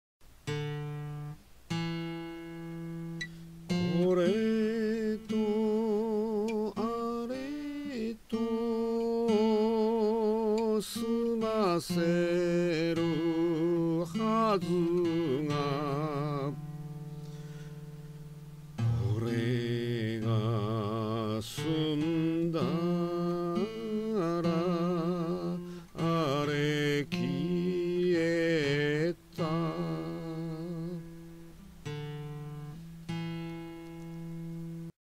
・・・あれ消えた
これとあれとを　済ませるはずが　これが済んだら　あれ消えた

最近、二つのことが一度にできなくなりました。クシュン